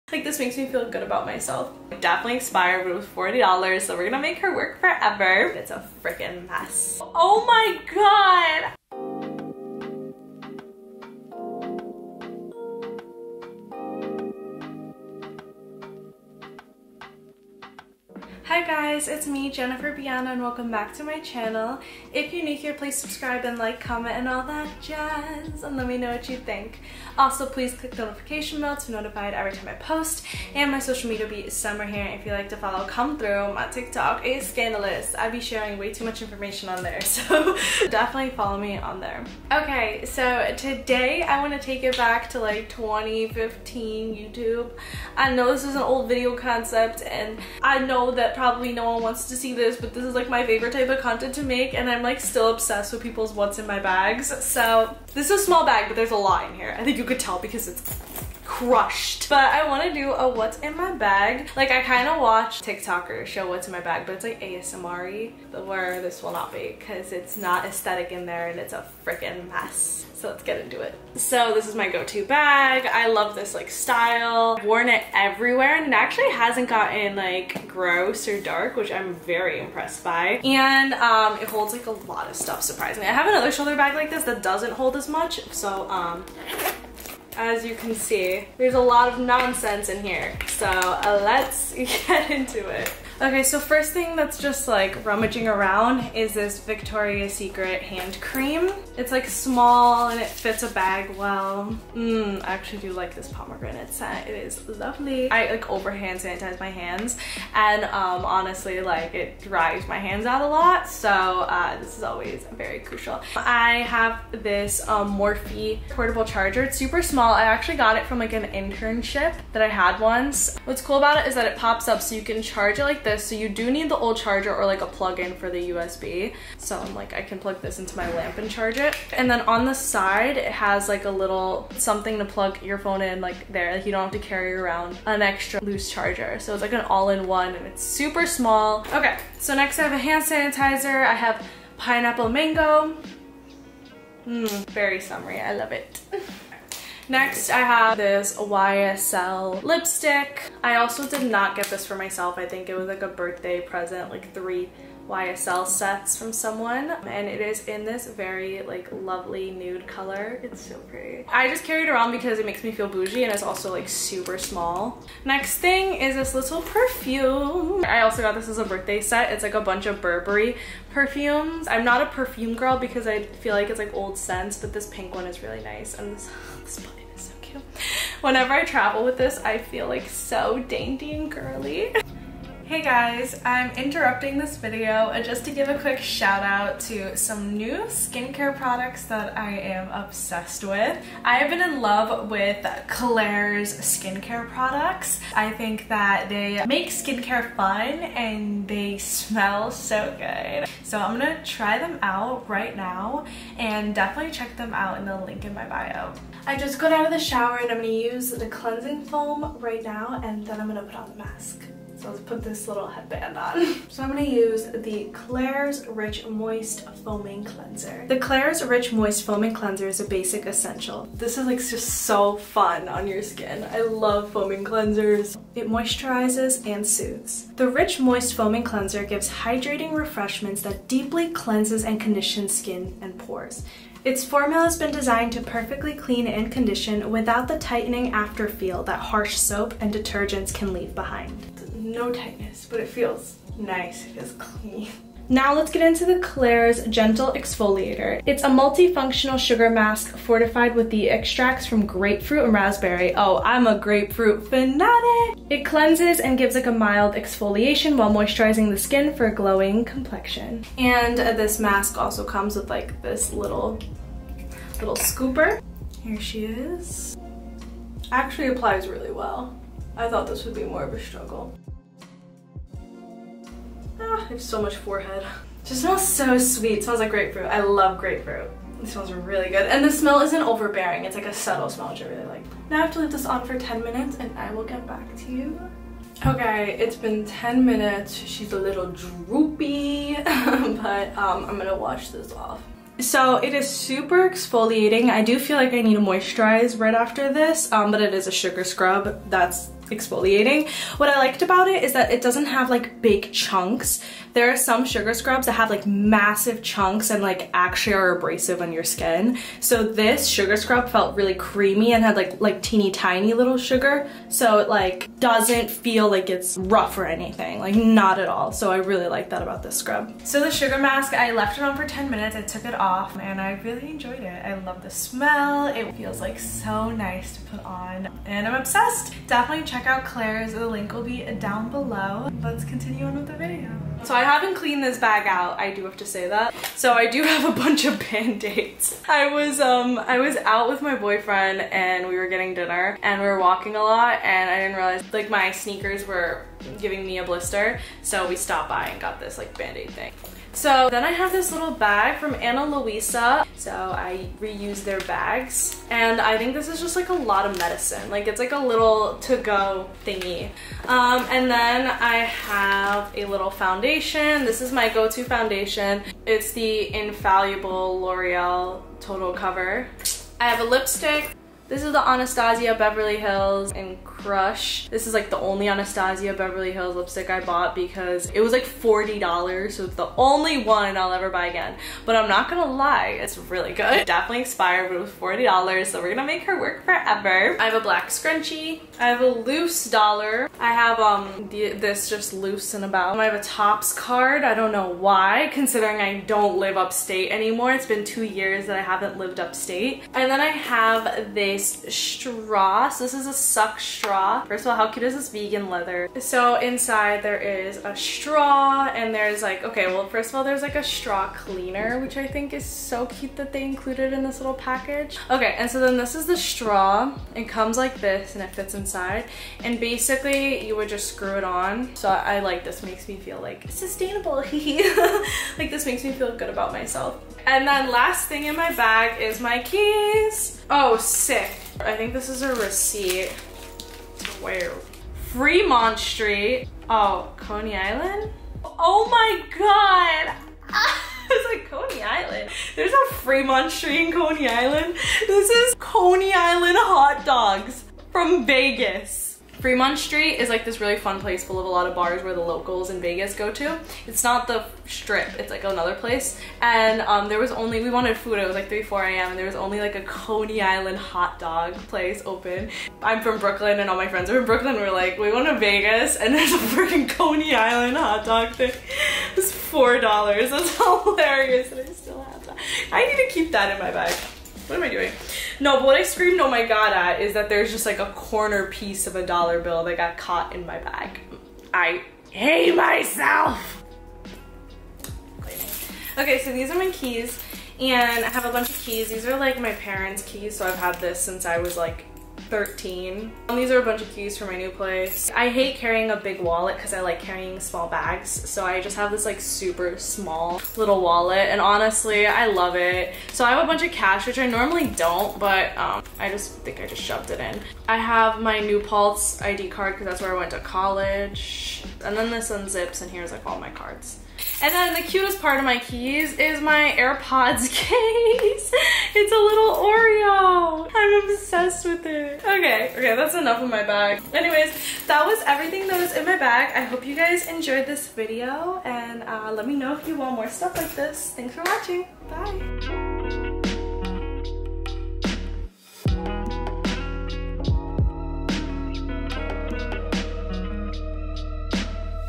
What's in my bag? Its all of a sudden 2015 Youtube again. why? bc i say so
This tiny lil shoulder bag carries A LOT! I love carrying cute mini items, it makes my heart happy